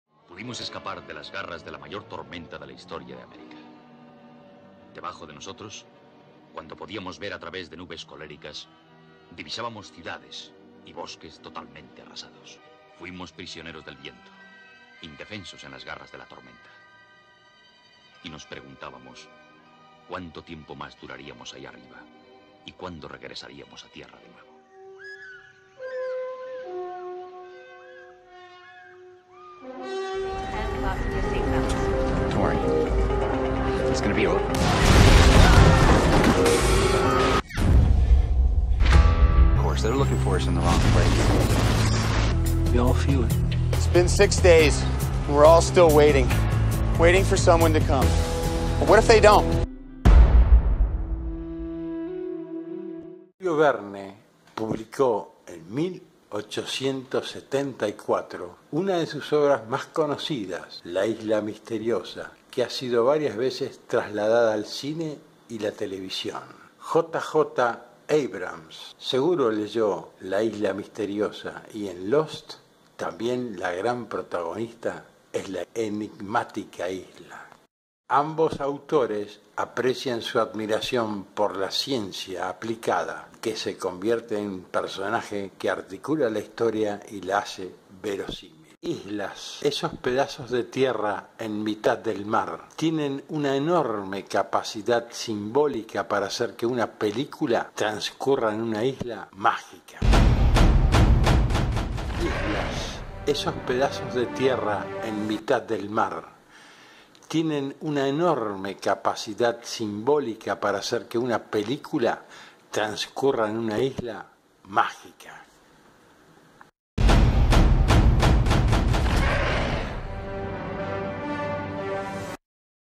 Julio Verne y J J Abrams
La Isla Misteriosa y LOST
Similitudes
Julio Verne, publicó en 1874 una de sus obras más conocidas y leídas, 'La isla misteriosa', que ha sido varias veces trasladada al cine y la televisión.
J.J. Abrams seguro leyó 'La isla misteriosa' y en 'LOST' también la gran protagonista es la enigmática isla.
Ambos autores aprecian su admiración por la ciencia aplicada, se convierte en personaje que articula la historia y la hace verosímil.
Primera parte:
En “La Isla Maravillosa” de Julio Verne un globo a la deriva que cae en una isla con 5 personas. En “Lost” de J.J. Abrams un avión cae en una isla y varias personas sobreviven.

Segunda Parte:
Los Sobrevivientes buscan salir de la isla. Fabrican una balsa, pero terminan en la isla.
Tercera parte:
El secreto de la isla. En una hay piratas que secuestran mujeres y en la otra están “los otros” que también secuestran mujeres.
 Al final, la isla desaparece completamente en el océano